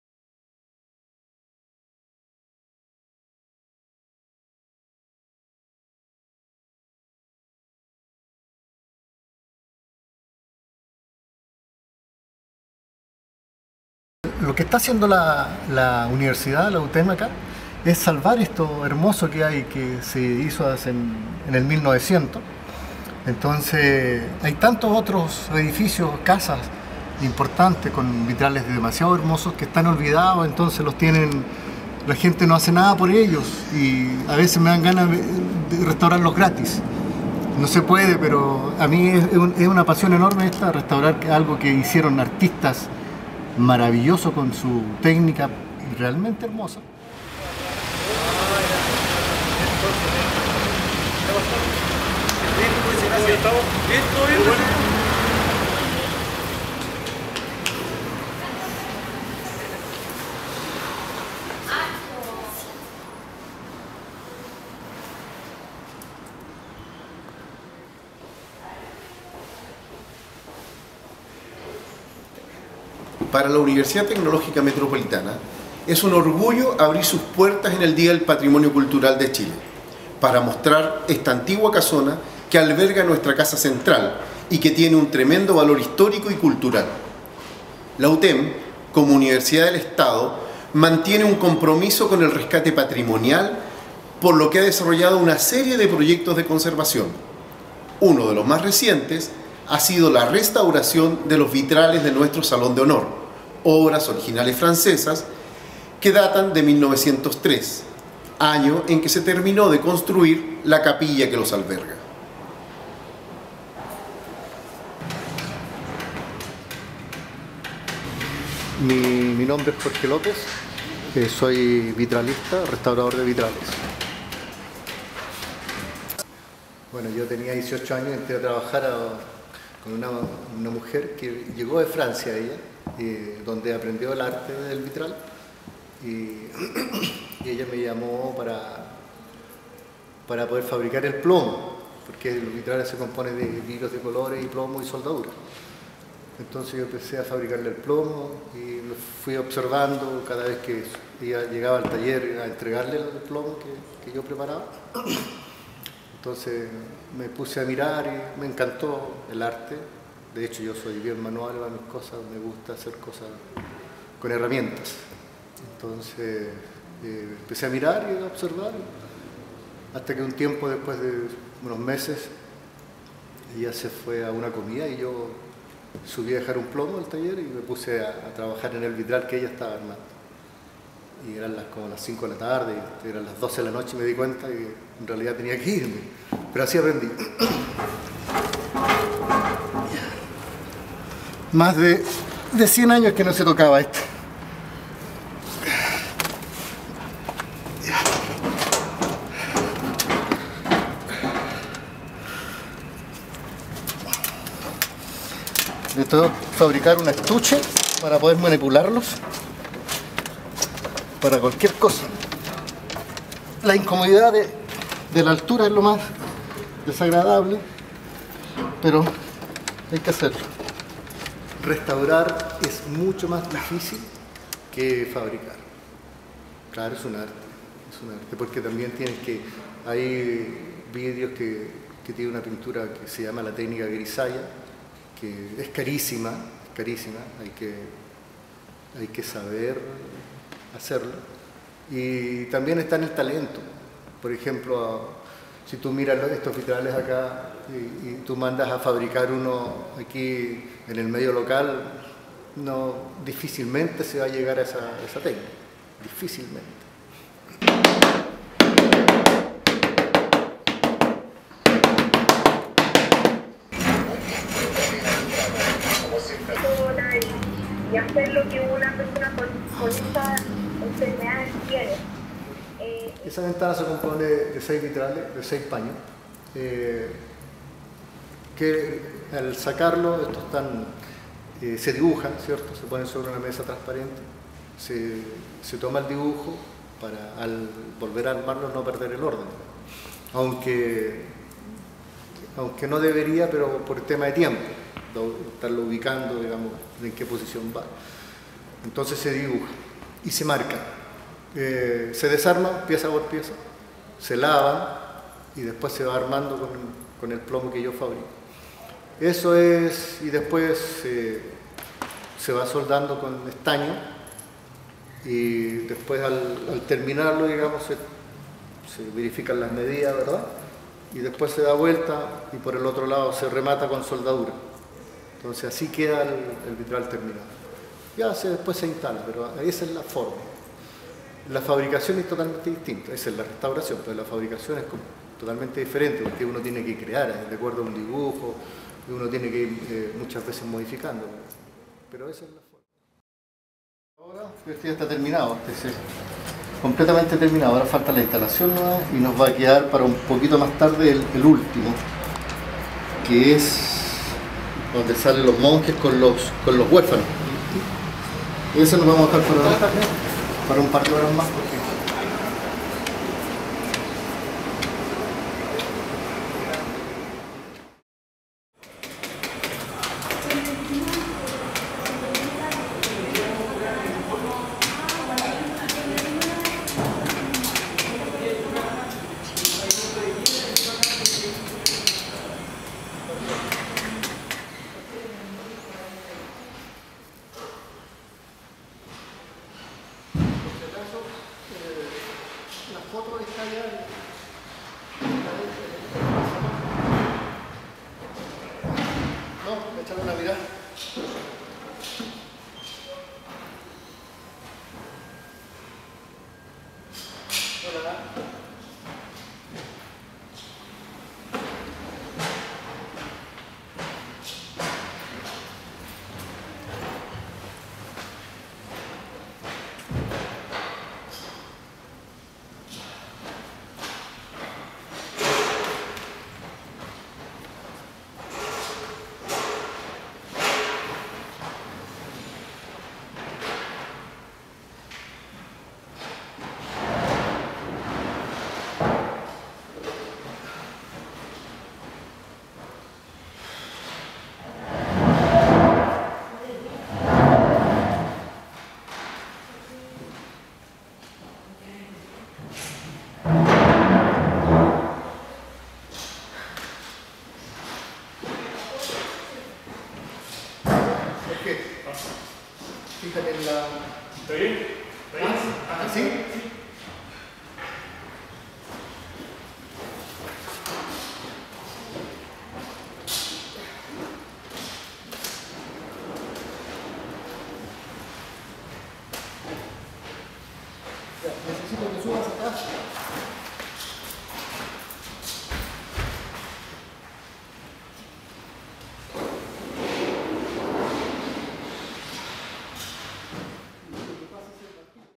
Restauración de vitrales de la Casa Central UTEM
El intenso terremoto que afectó a la zona central del país el año 2010, generó varios daños a los hermosos vitrales del artista Lucien Bégule que se encuentran al interior de la antigua capilla (hoy Salón de Honor) de la Casa Central de la Universidad Tecnológica Metropolitana, que datan del año 1903.

La Universidad, comprometida con el patrimonio, inició la restauración de los vitrales el año 2013, finalizando el 2014.

El responsable de restaurar cada uno de los vitrales es Jorge López, restaurador con una larga trayectoria en este arte quien se lamenta que existan pocos interesados en aprender sobre su disciplina: “Restaurar es mucho más difícil que fabricar. Es un arte”